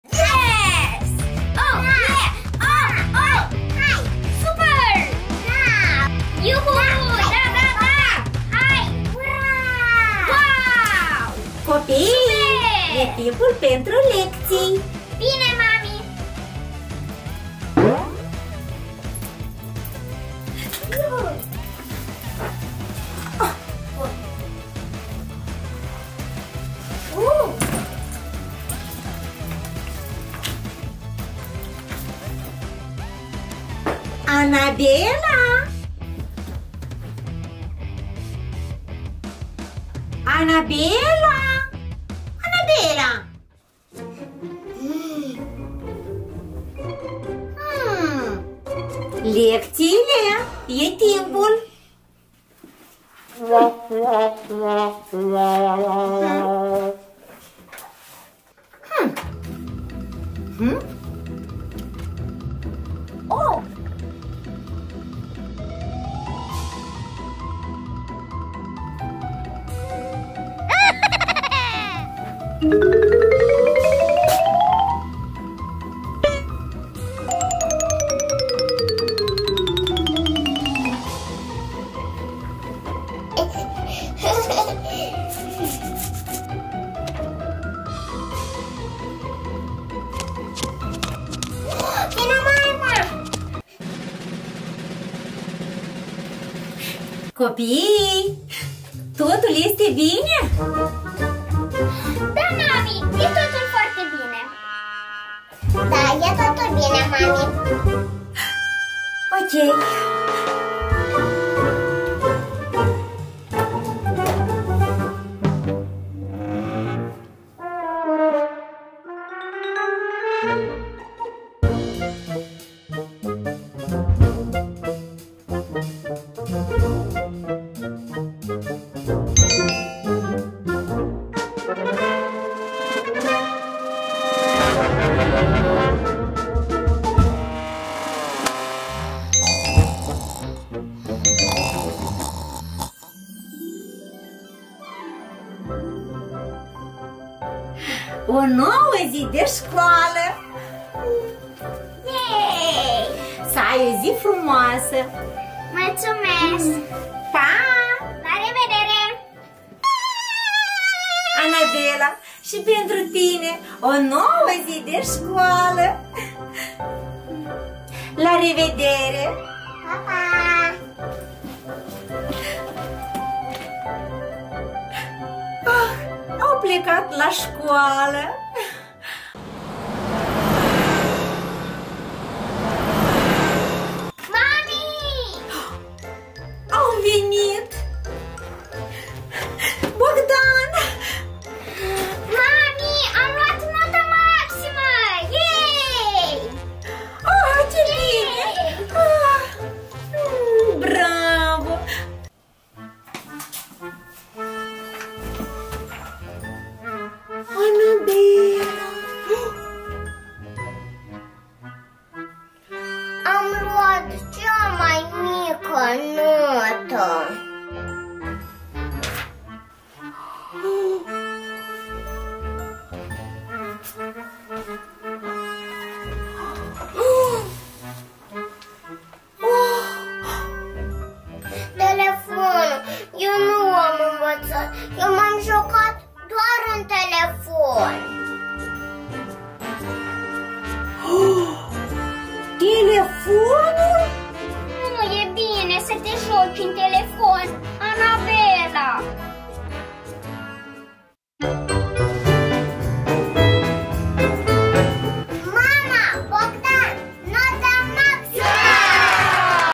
Bogdan Learn Math but Anabella Play on Phone | Educational Video for Children
Bogdan Pretend Play Learn Math but Anabella Play on Phone | Educational Video for Children.  This  educational video for children will teach them that it is good to study and do homework or else they might do bad in school. Anabella  learns that  is very bad to play on phone and she should focus on studying and learning.

👦🏼 Bogdan Show Channel:  @BogdansShow
👧 Anabella Show Channel:  @AnabellaShow
👩🏻‍🦳 Anabella Show EN:  @AnabellaShowEN